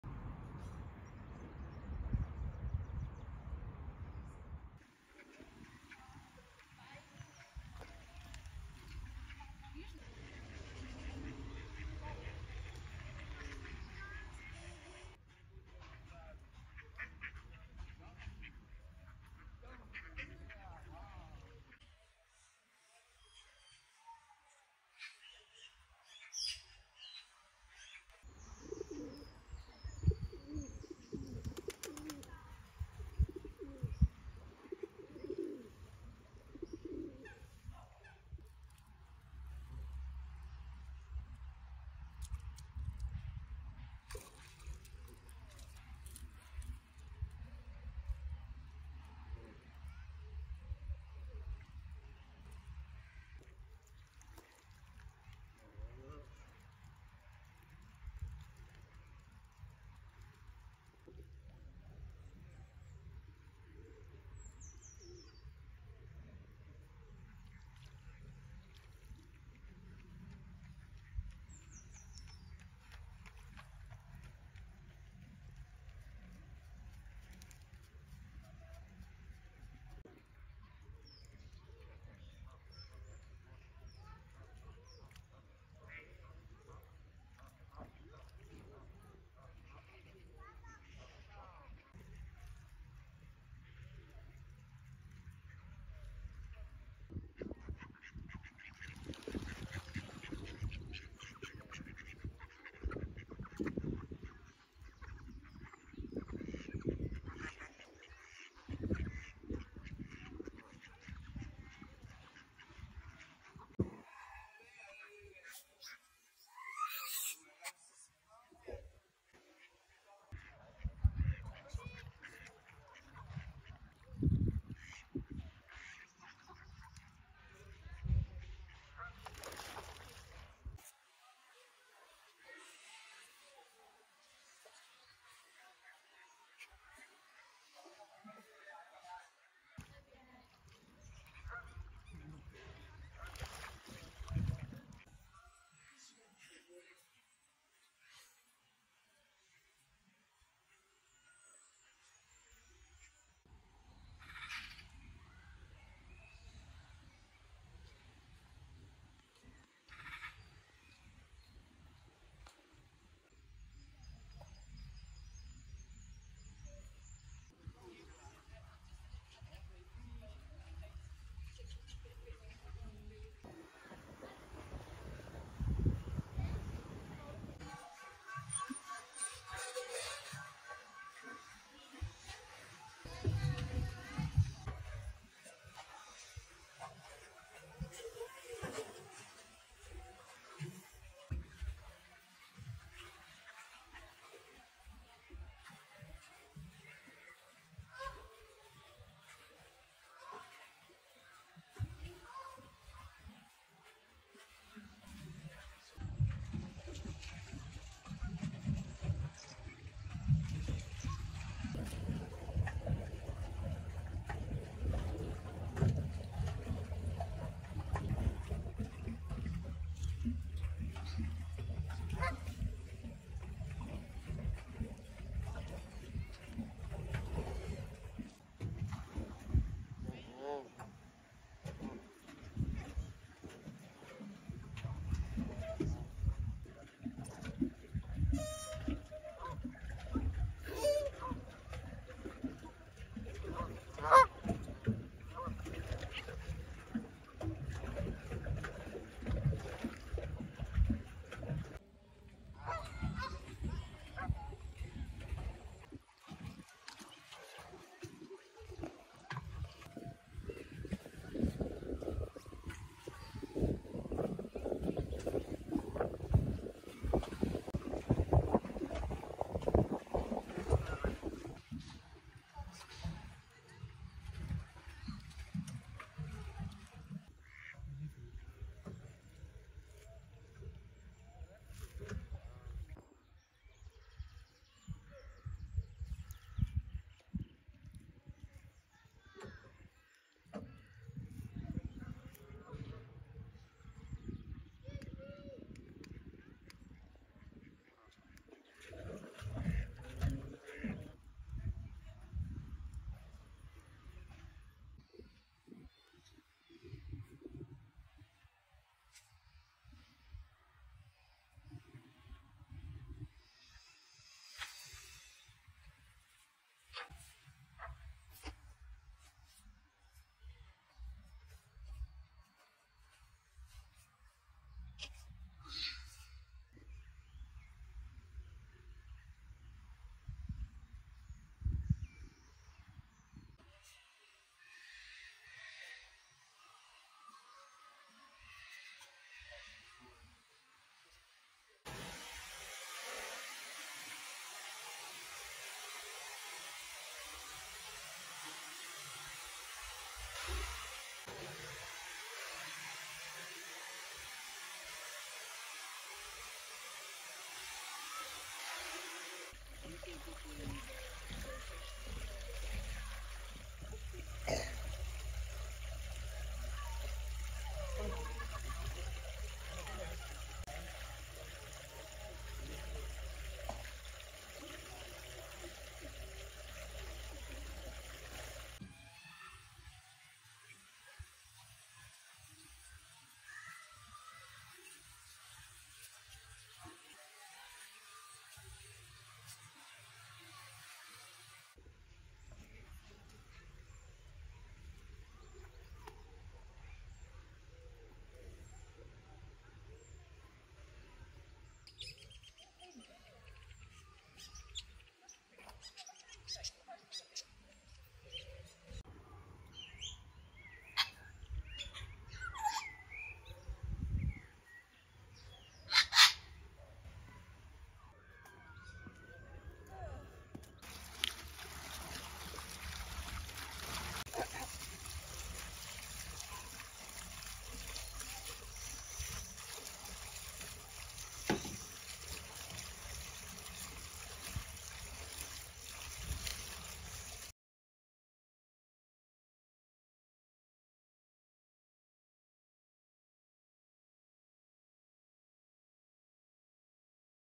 Peaceful relaxing nature / Silent vlog
Hello everyone, these are the videos I have put together from a recent vacation. I hope you enjoy it!